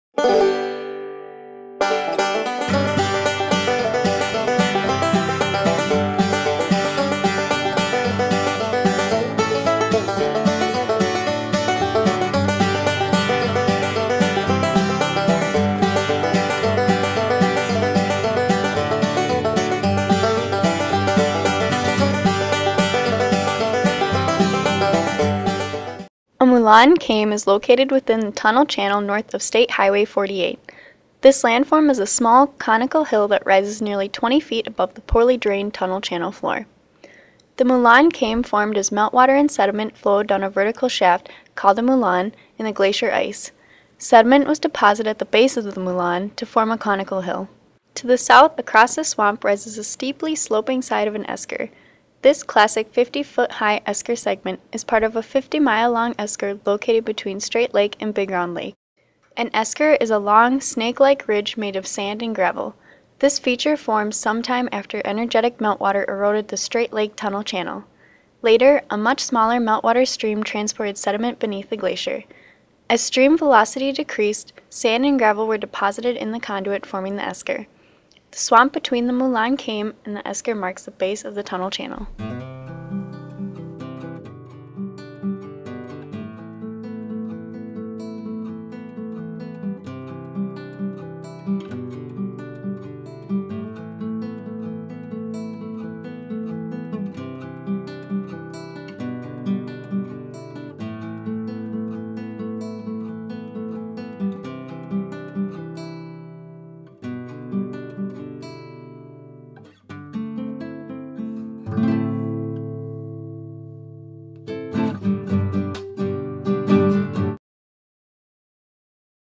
Moulin Kame and Esker (hiking guide stops #7 & 8)
Straight Lake State Park, Polk County, Wisconsin
Geology Hiking Guide for the Ice Age Trail

Ian F. Freeman, Rebecca J. Moore,
and Kent M. Syverson

University of Wisconsin-Eau Claire Department of Geology

Captions:

A moulin kame is located within the tunnel channel north of State Hwy 48. This landform is a small conical hill that rises nearly 20 feet above the poorly drained tunnel channel floor.
The moulin kame formed as meltwater and sediment flowed down a vertical shaft, called a moulin, in the glacier ice.   Sediment was deposited at the base of the moulin to form a conical hill.
To the south across the swamp rises the steeply sloping side of an esker.  This classic 50-ft-high esker segment is part of a 5-mile-long esker located between Straight Lake and Big Round Lake.

An esker is a long, snake-like ridge made of sand and gravel. This feature formed sometime after energetic meltwater eroded the Straight Lake tunnel channel.  Later, a much smaller meltwater stream transported sediment beneath the glacier.  As stream velocity decreased, sand and gravel were deposited in the conduit forming the esker. The swamp between the moulin kame and the esker marks the base of the tunnel channel.

At this stop the trail follows a classic sharp-crested esker segment north of State Hwy 48.  This 50-ft-high esker segment is part of a 5-mile-long esker located between Straight Lake and Big Round Lake.

An esker is a long, snake-like ridge made of sand and gravel. This feature formed sometime after energetic meltwater eroded the Straight Lake tunnel channel.  Later, a much smaller meltwater stream transported sediment beneath the glacier.  As stream velocity decreased, sand and gravel were deposited in the conduit forming the esker. The lake and wetlands on either side of the esker at this stop mark the base of the tunnel channel.